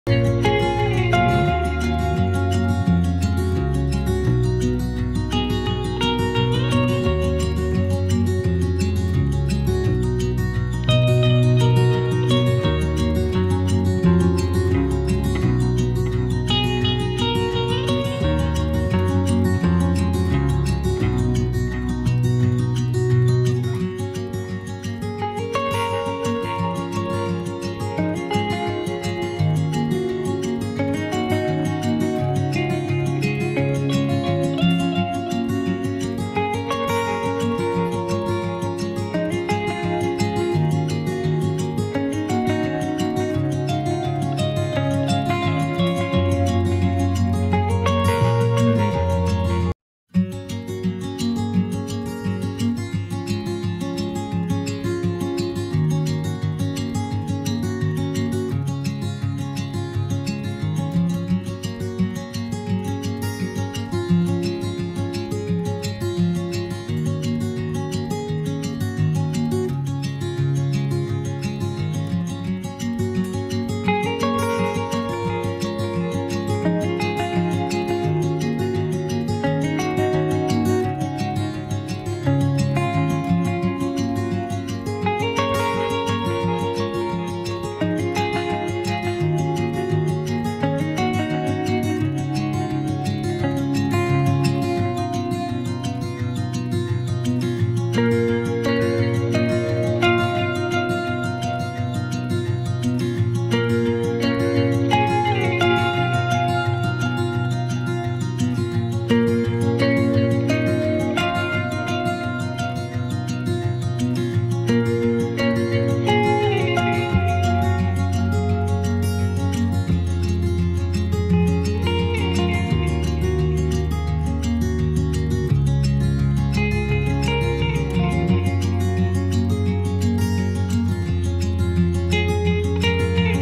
NTR Looks Towards Janhvi Kapoor Visuals at Devara Movie Opening Ceremony | Jr NTR and JanhviKapoor
Watch : NTR Looks Towards Janhvi Kapoor Visuals at Devara Movie NTR30 Opening Ceremony | Jr NTR and JanhviKapoor Videos